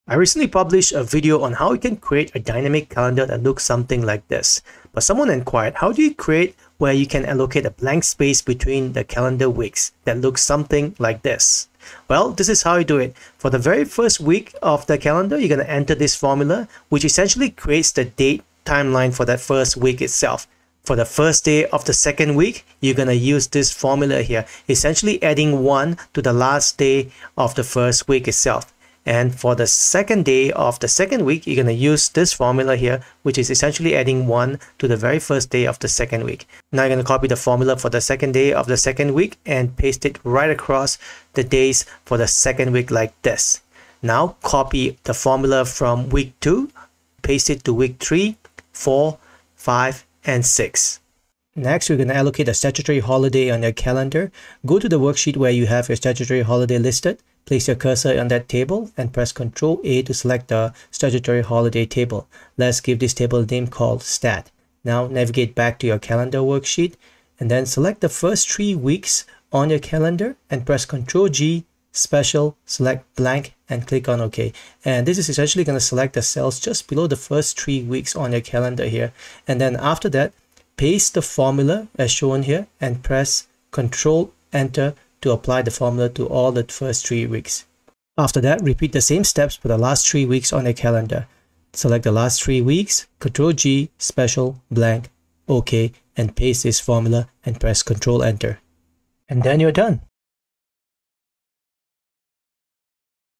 Dynamic Monthly Calendar With Statutory Holiday In Excel - Excel Tips and Tricks
Learn how to create dynamic monthly calendar with Statutory Holiday in Excel.

Creating an automatic monthly calendar in Excel is a straightforward process that begins with setting up the basic structure. Start by opening a new Excel workbook and selecting a blank worksheet. Then, establish the layout of your calendar by creating headers for the days of the week, followed by entering the dates for the corresponding days. To ensure your calendar updates dynamically, you may wish to explore Excel's built-in templates or utilize manual input methods. Additionally, Excel provides various functionalities for automating tasks, such as automatically adding monthly dates and creating recurring events. If you're looking to incorporate holidays into your calendar, you can input them manually or explore external sources for holiday calendar templates that can be integrated into your Excel sheet. Ultimately, by following these steps, you can easily create a comprehensive and functional monthly calendar in Excel without relying on complex formulas or recommendations.